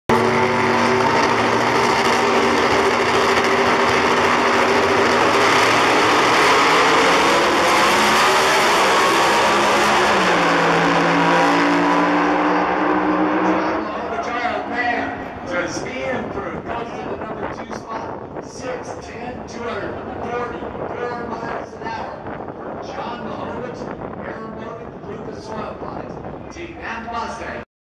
NMCA POMONA JOHN MIHOVETZ JOHN SCIALPI MAY 17 2014
JOHN MIHOVETZ RUNS 6.10 @ 244.25 MPH
JOHN SCIALPI RUNS 6.323 @ 220.08 MPH